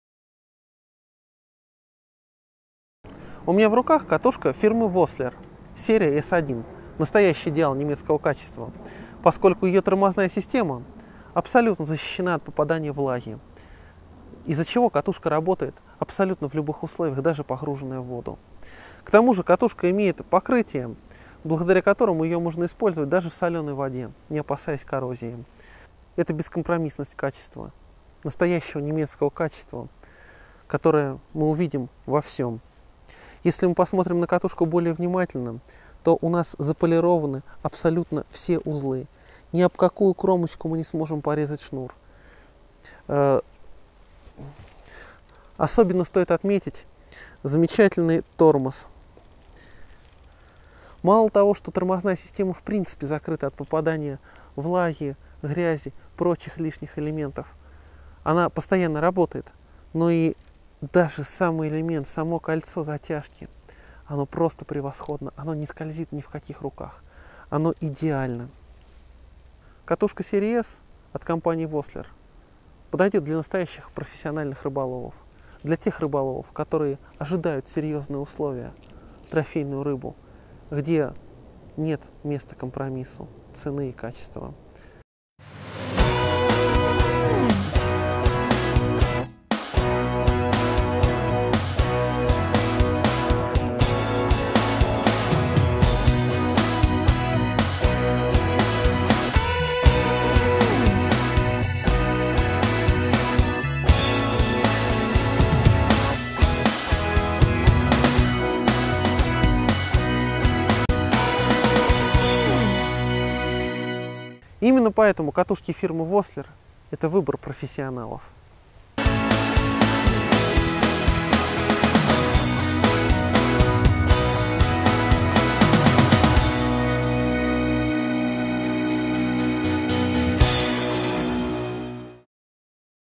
Катушка Vosseler
Абсолютная надежность. Закрытая тормозная система. Возможность использования для рыбалок в море. Эталон немецкого качества и стиля. Бескомпромиссный выбор.